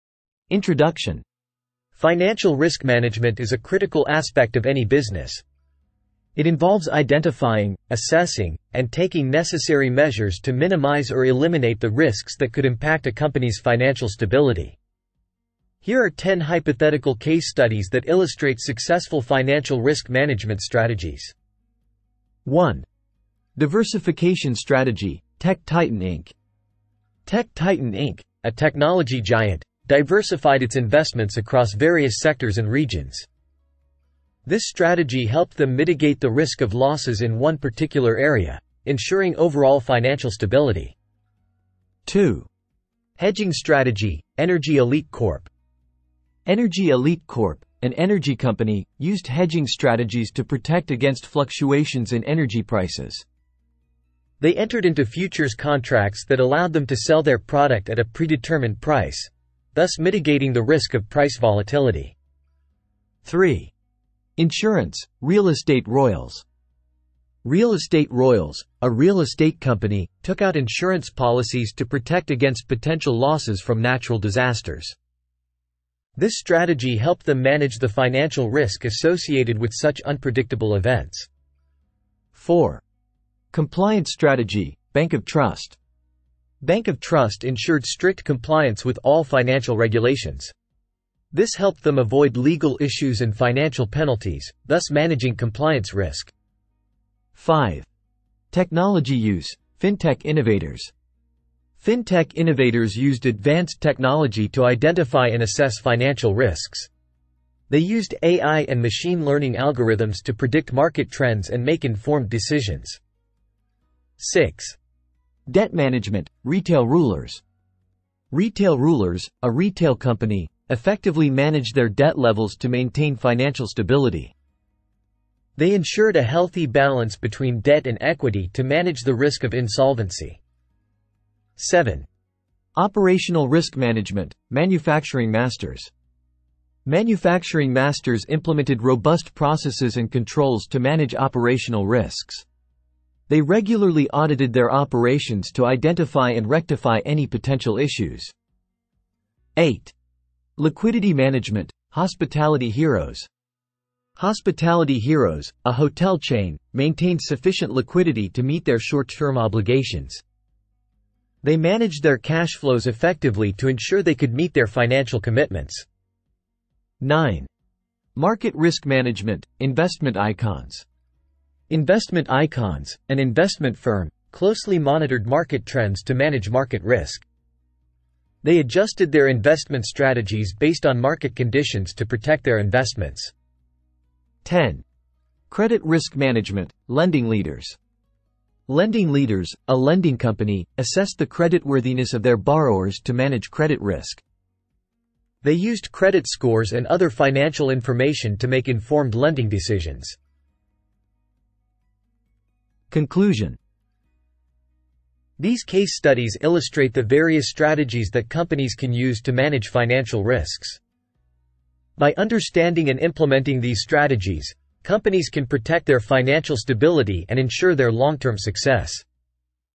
Top 10 Case Studies on Successful Financial Risk Management Strategies
Dive into real-world case studies showcasing successful financial risk management strategies implemented by organizations across diverse industries, providing valuable insights into effective risk identification, assessment, and mitigation practices.